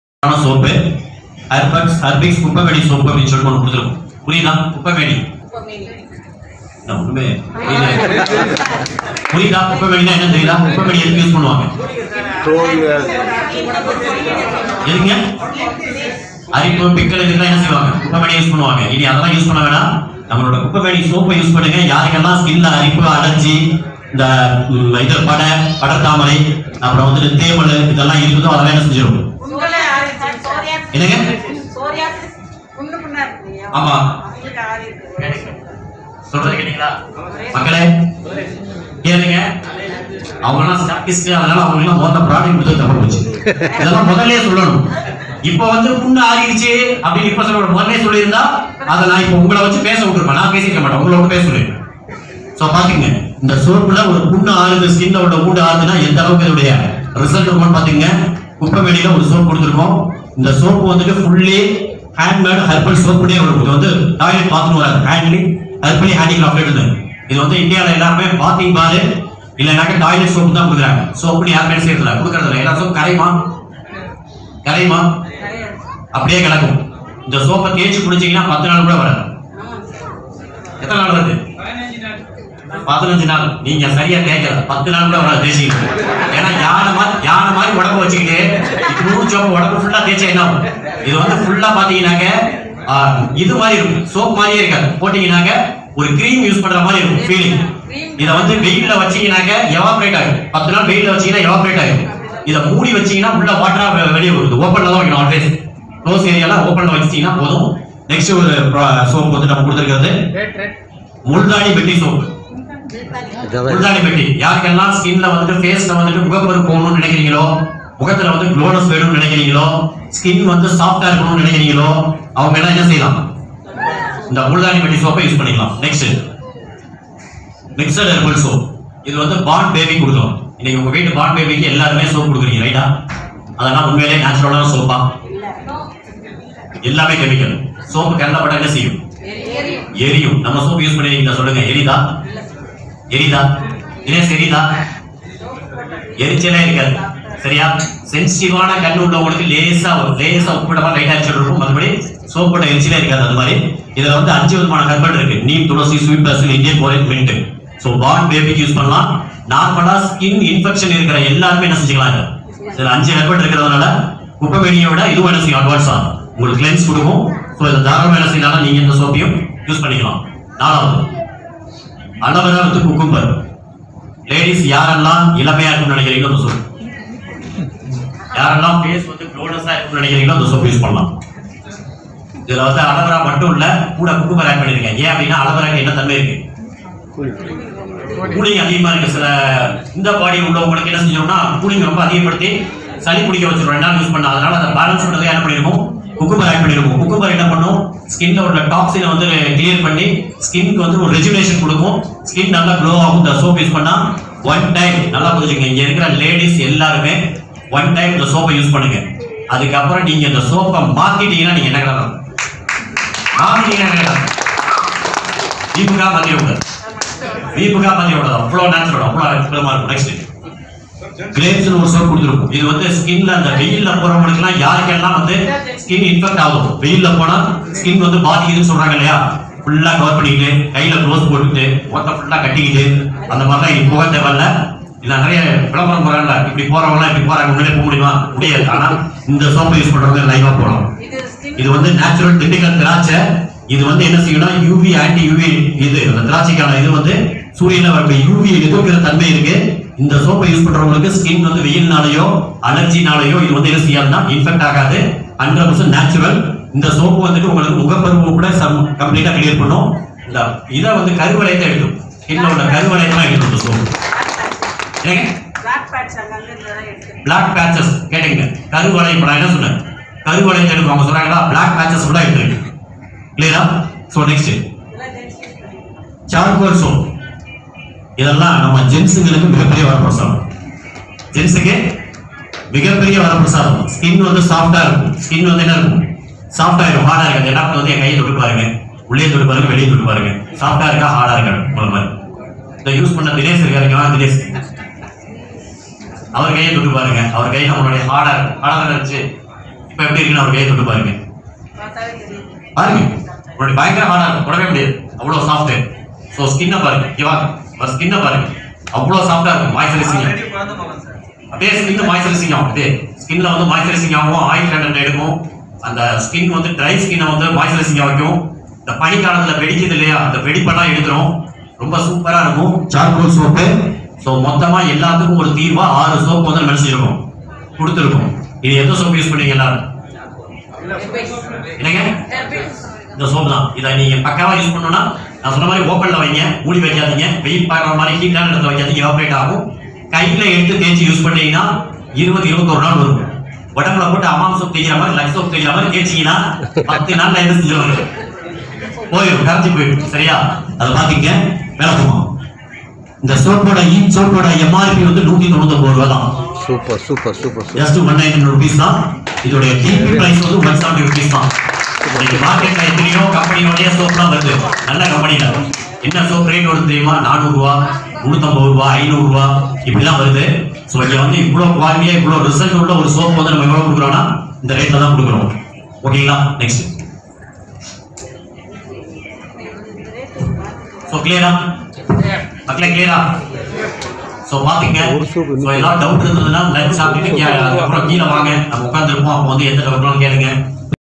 ஸ்கின்னுக்கு ஏற்ப தனித்துவமான பாத்திங் சோப், நம்ம குழந்தைக்கு யூஸ் பண்ணலாம் . FREE JOIN
1  KUPPAIMENI SOAP - குப்பாமேனி சோப்
2. GRAPES SOAP  - கிராப்ஸ் சோப்
3. ALOW VERA WITH CUCUMBER SOAP- அலோ வேரா சோப் வெள்ளரி சோப்புடன்
4. MULTHANI METTI SOAP -முல்தானி மெட்டி சோப்
5. MIXED HERBAL SOAP -5 ஹெர்பல் கலந்த  சோப் ( NEEM+TULSI+SWEET BASIL+INDIAN BORAGE+MINT)
6. CHARCOAL SOAP
 (  இன்று கடையில் இயற்கைனு சொல்லியிருந்த பல சோப்புகள்ல கெமிக்கல் கலந்திருக்கிறதை சின்ன சின்ன எழுத்துகள்ல அவங்களே போட்டிருந்தாங்க. கெமிக்கல் ஃப்ரீனு போட்டிருந்ததுலகூட பல கெமிக்கல்ஸ்)
👉 உங்கள் குடும்பத்தில் பொருளாதாரம் மற்றும் உடல் ஆரோக்கியத்திற்கு முழுமையான தீர்வு காண இதோ அற்புதமான  வாய்ப்பு உங்களுக்காக காத்திருக்கிறது!